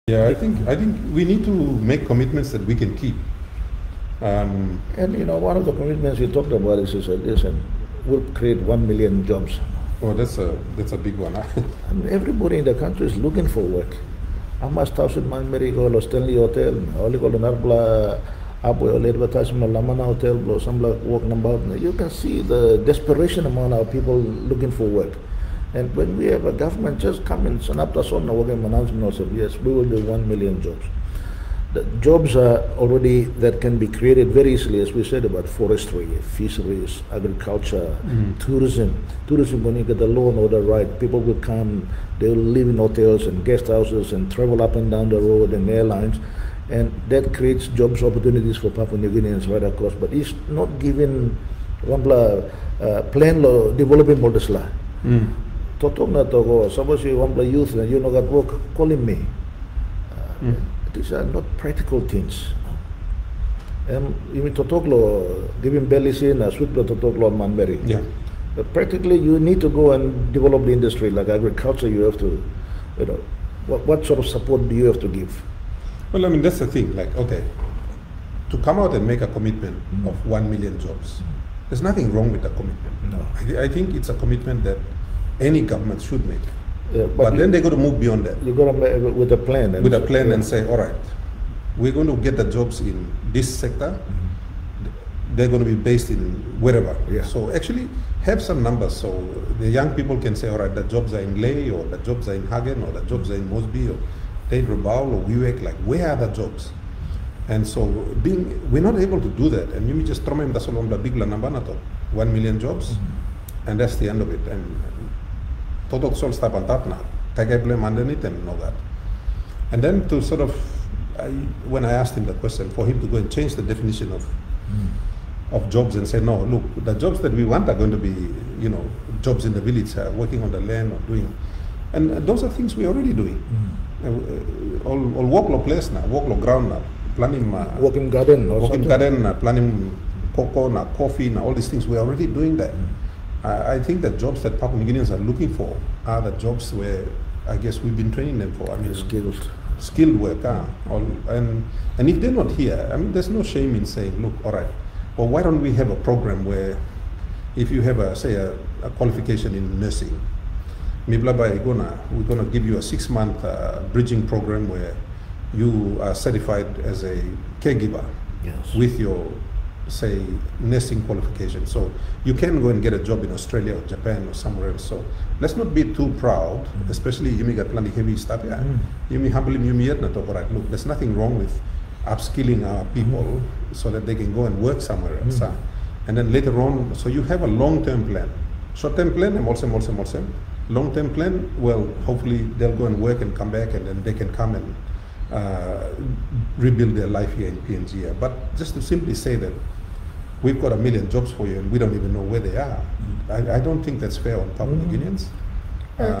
Hon Allan Bird and Hon Peter O'Neill on Jobs in PNG
Full video can be found on Hon. Peter O'Neill's Facebook page